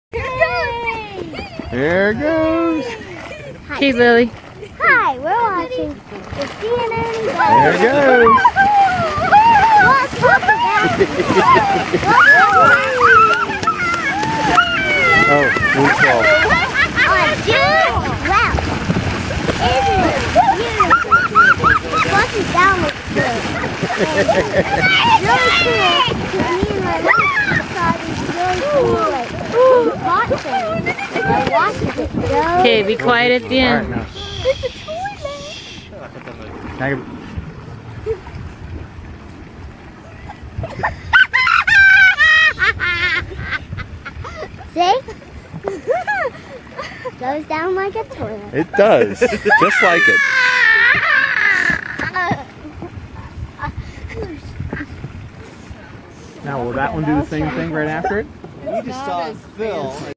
Toilet Geyser
HYSTERICAL LAUGHTER AT GEYSER ERUPTION.  DISTURBING.

Anemone Geyser, Yellowstone.